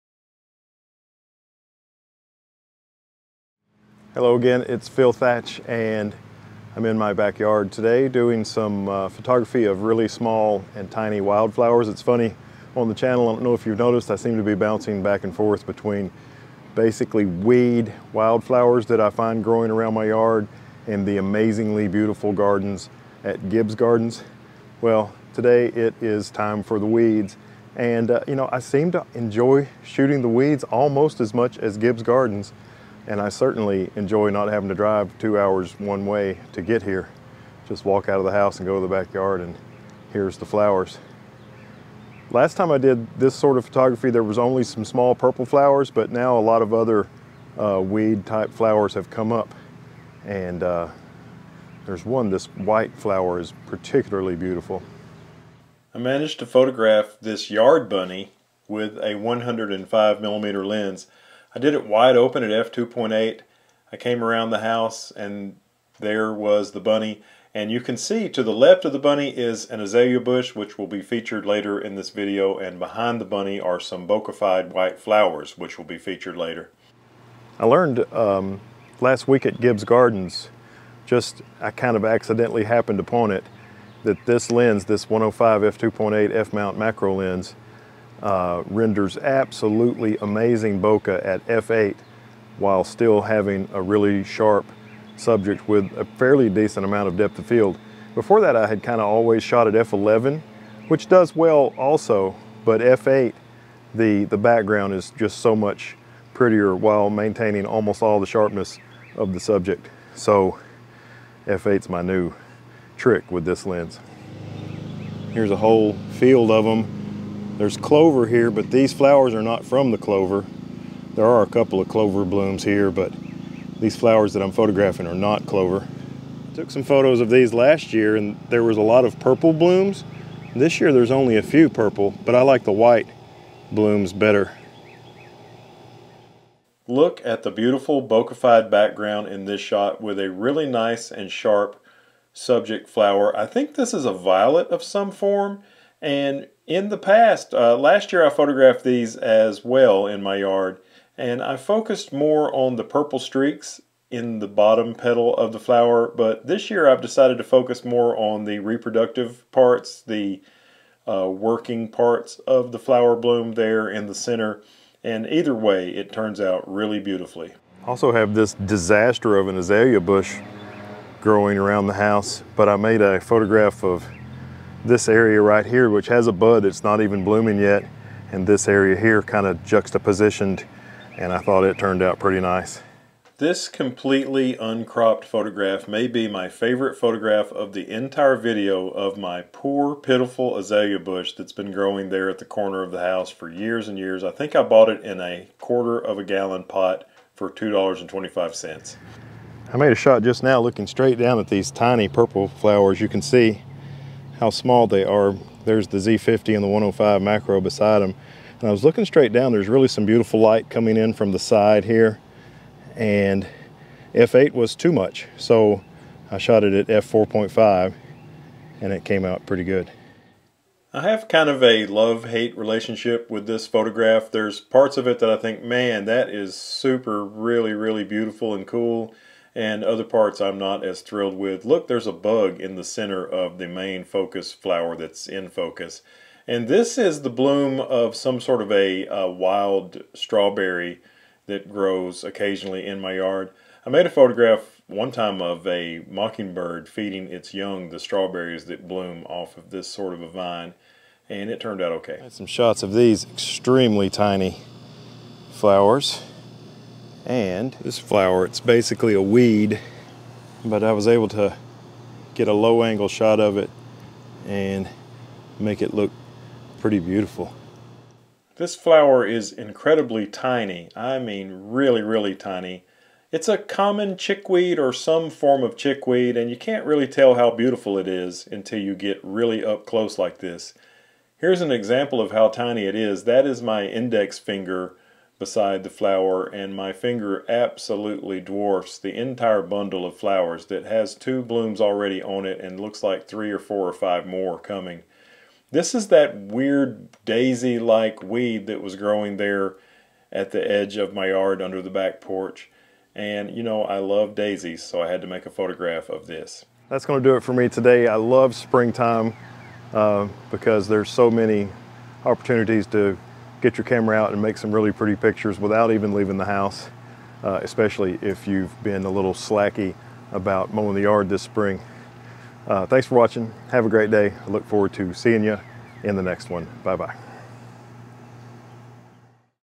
Nikon Z50 • Tiny Flower Macros and a Rabbit! 105mm 4K
Using my Nikon Z50 and the 105mm f/2.8 "Micro" macro lens, I make photos of tiny white violets and more in my yard.